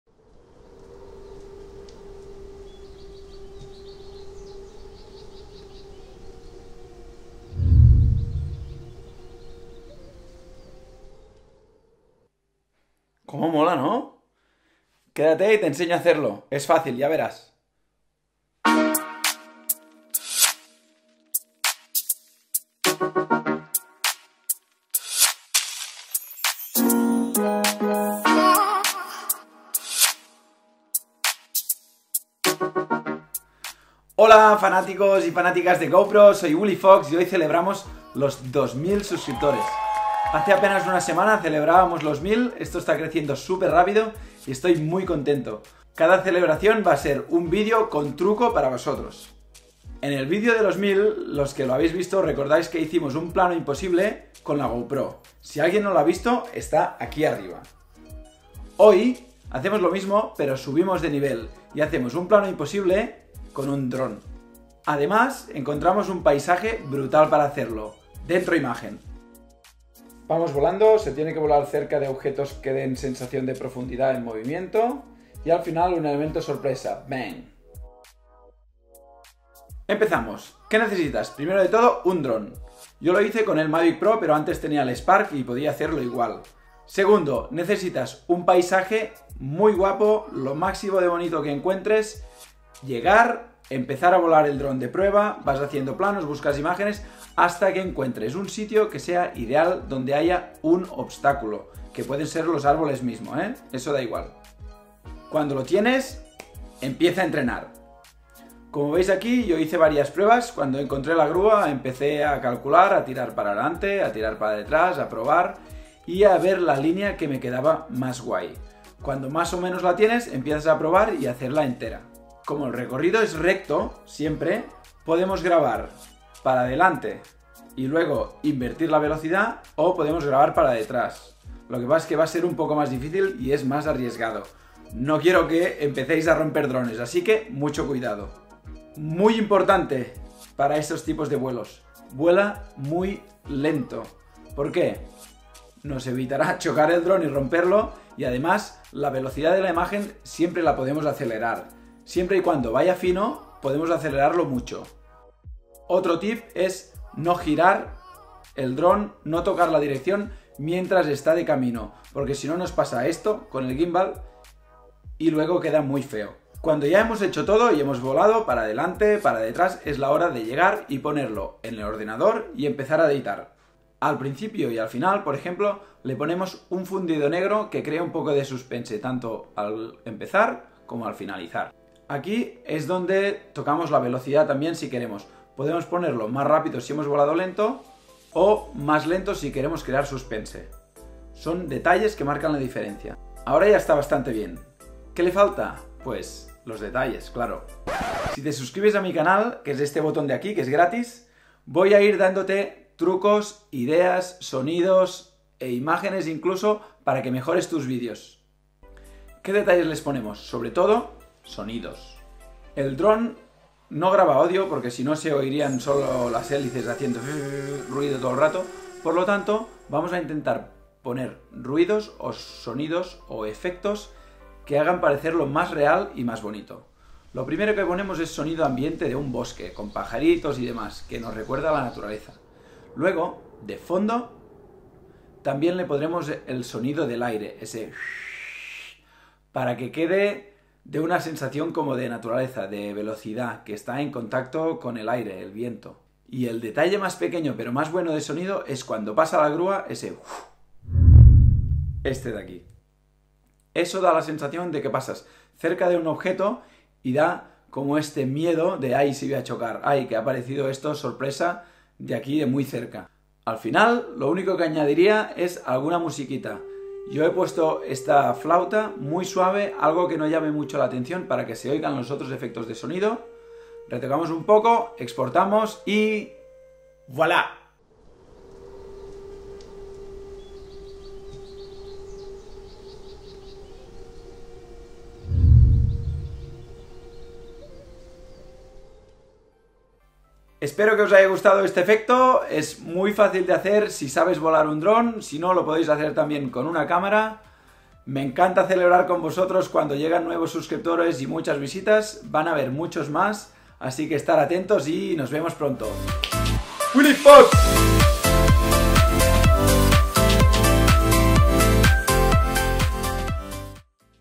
🚁Plano IMPOSIBLE con DRON! Mavic Pro (especial 2000 subs)
Cómo hacer un PLANO IMPOSIBLE con tu dron?!?! Te enseño todo el proceso MUY FÁCIL con un resultado increíble! Un plano super bonito con un DJI Mavic pro que puedes hacer con cualquier dron...o incluso con una Gopro Hero 7 black por ejemplo si quieres!

Es el truco que os enseño para CELEBRAR que ya somos 2.000 SUSCRIPTORES en el canal! Sólo una semana después de alcanzar los 1.000!